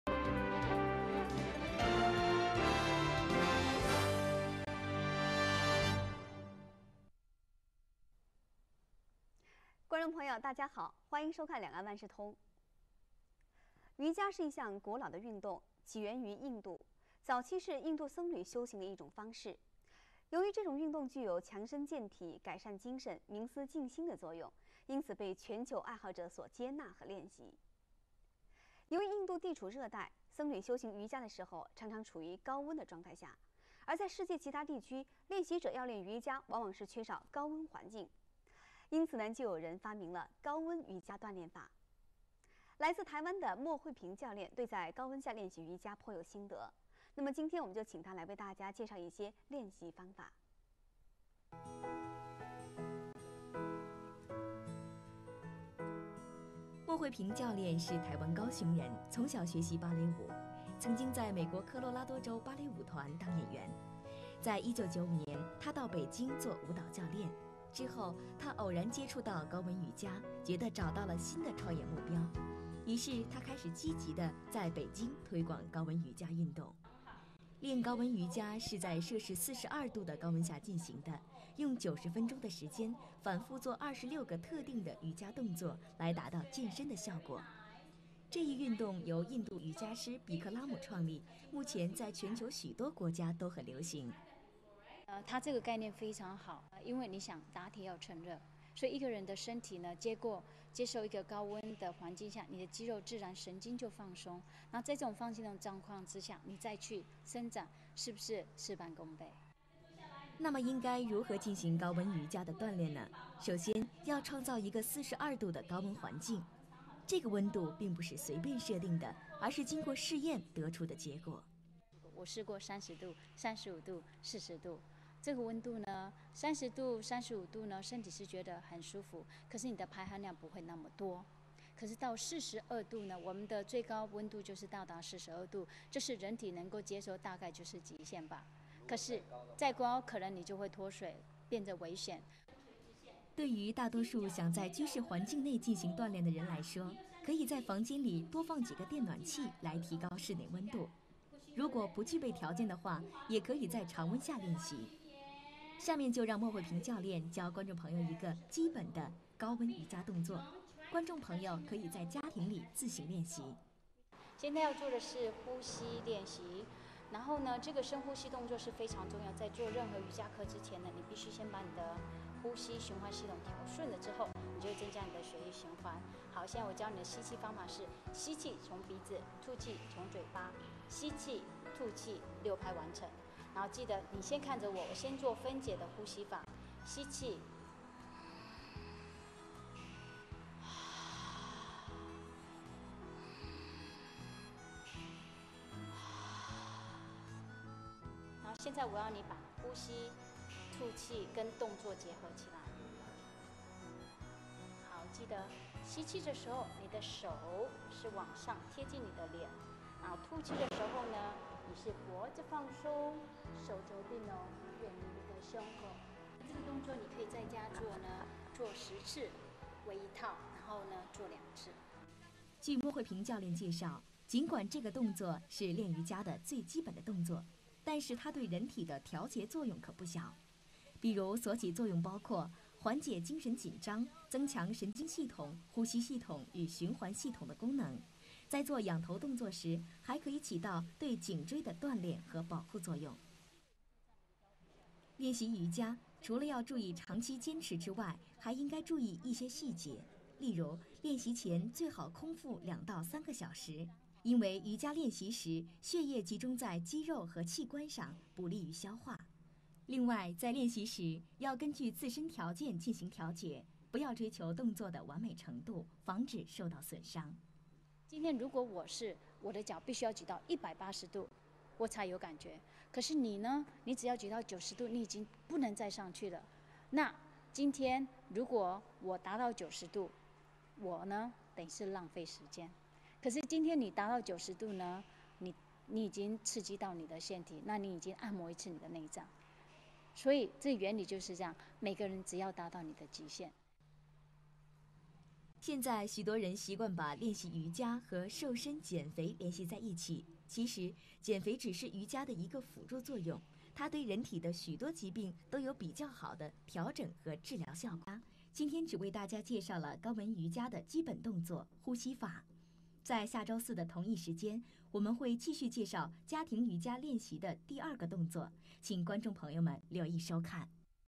Huiping Mo Yoga Demonstration On Chinese TV 莫慧萍，中国，热瑜珈，舞蹈，芭蕾
Huiping gives a yoga demonstration on a Beijing Television program.